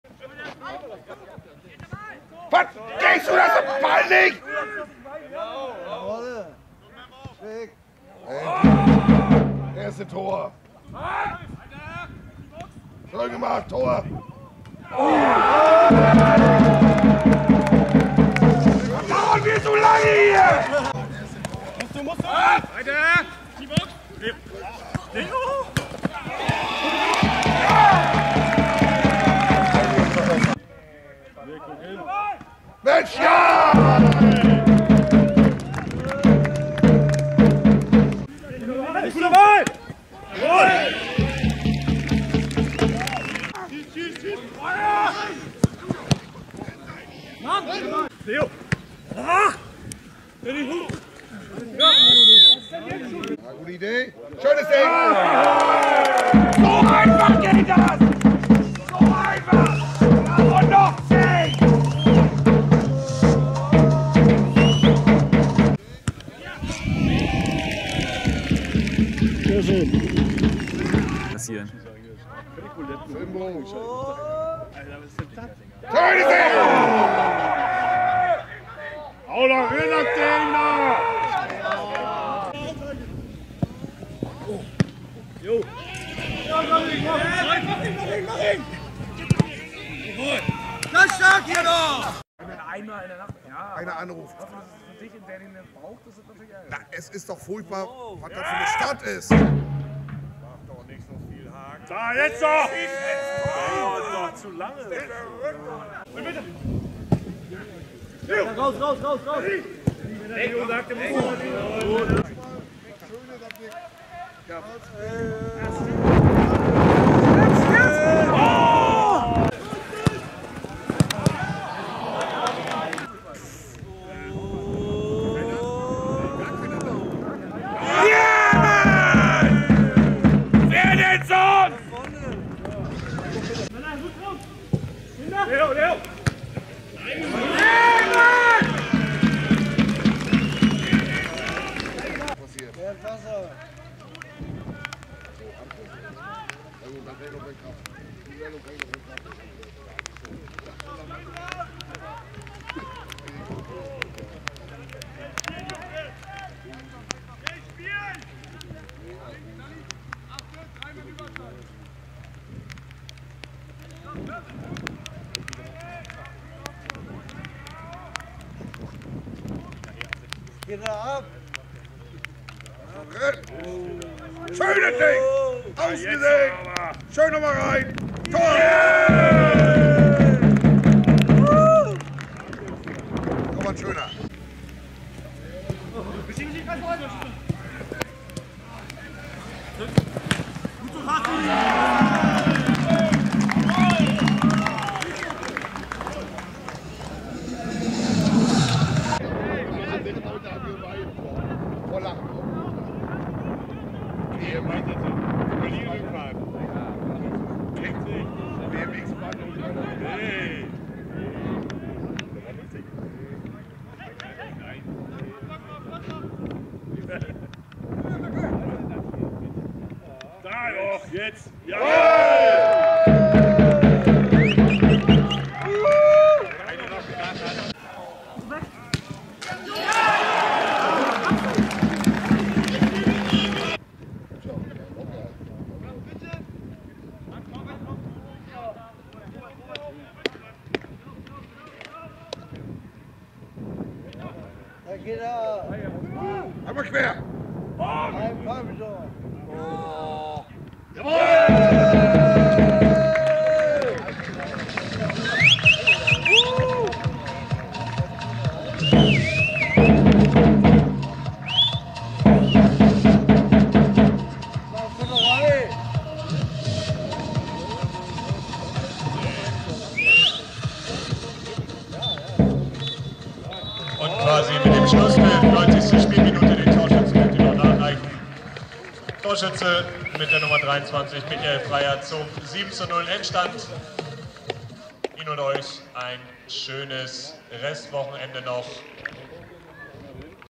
1.FC Neubrandenburg 04 - SV Warnemünde 7:0 (8.10.2022)
Tore:
0:00 1:0 Christoph Fischer (2.)
0:30 2:0 Lukas Ebel (13.)
0:53 3:0 Daniel Nawotke (23.)
2:03 4:0 Christoph Fischer (68.)
2:44 5:0 Lennard Fassinger (73.)
3:33 6:0 Joke Reichel (88.)
3:53 7:0 Michael Freyer (90.)